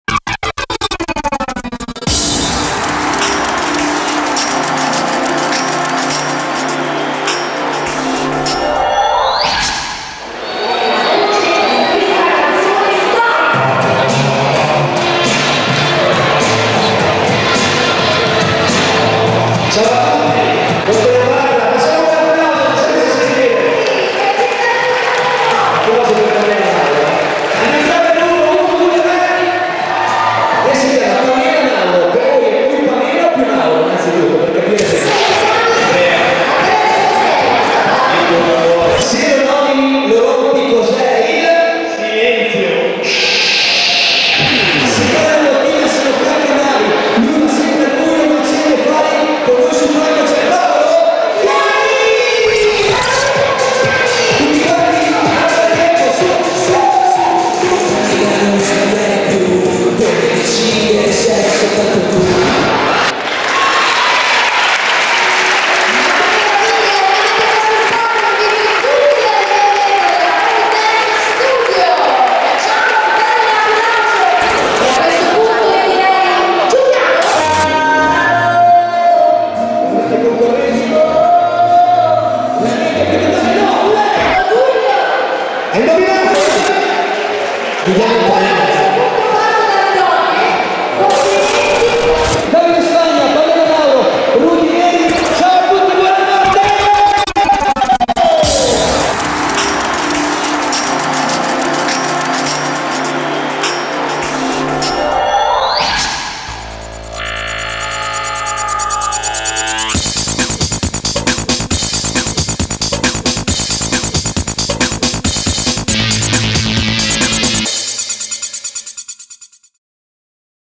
Show Reel - Pamela Mauro & Rudy Neri
Dj con musica, giochi con il pubblico gadget e premi x i vincitori!
* Gara di canto
* Gioco - Indovinelli
* Gara ballo
* Gioco - Indovina il personaggio misterioso
* Concorso miss centro commerciale

Il compenso comprende:
- Presentatrice e Animatore
- DJ "Tecnico audio"
- Impianto audio e luci

momenti di una serata live in un centro commerciale!